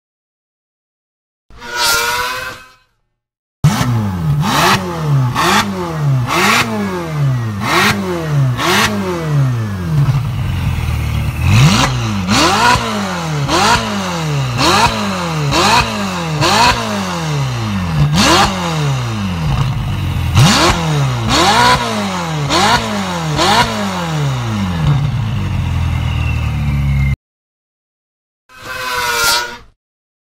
Ferrari 488 Spider X Fi EXHAUST
Fi EXHAUST adds that extra “R” sound to Ferrari. FeRRRaRi! 🏎️

Thanks to Professional Auto Repair and Customization Shop @Signature.Autohaus on Instagram